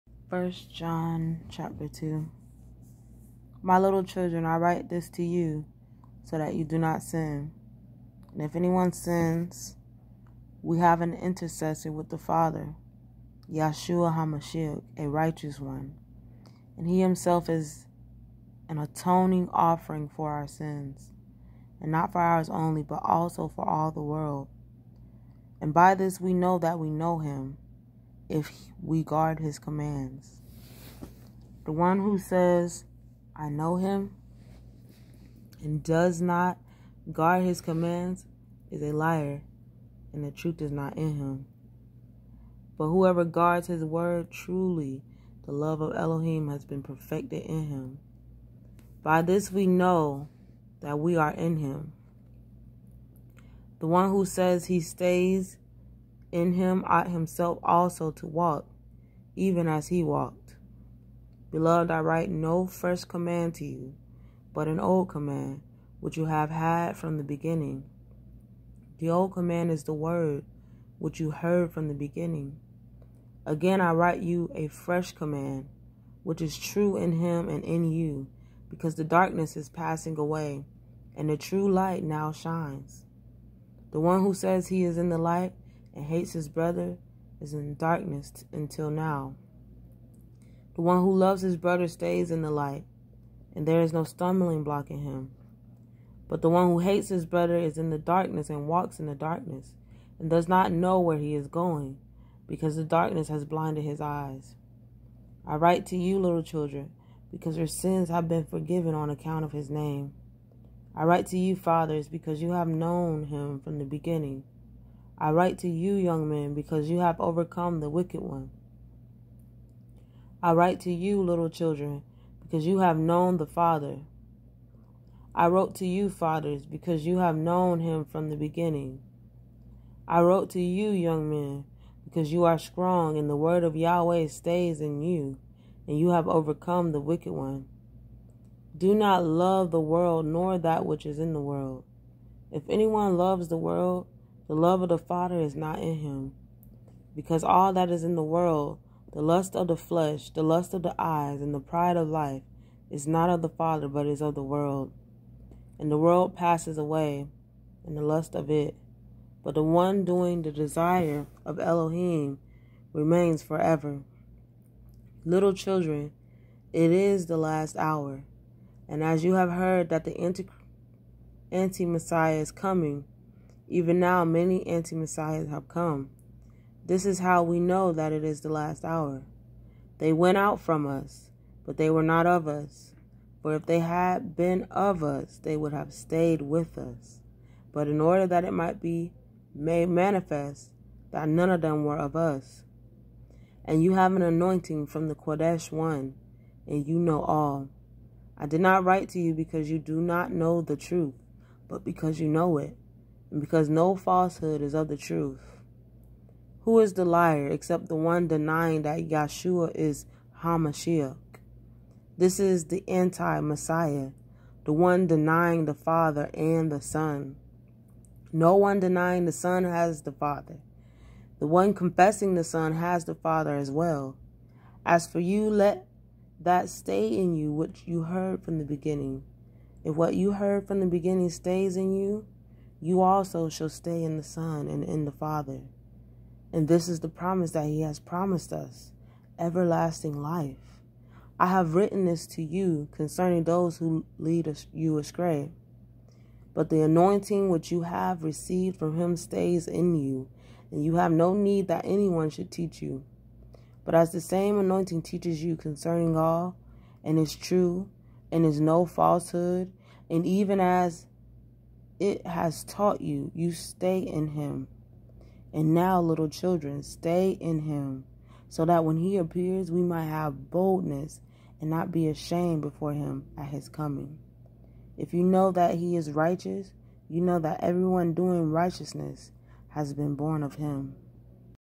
1John 2
Reading aloud the 2nd chapter of 1John  from the halleluYah scriptures.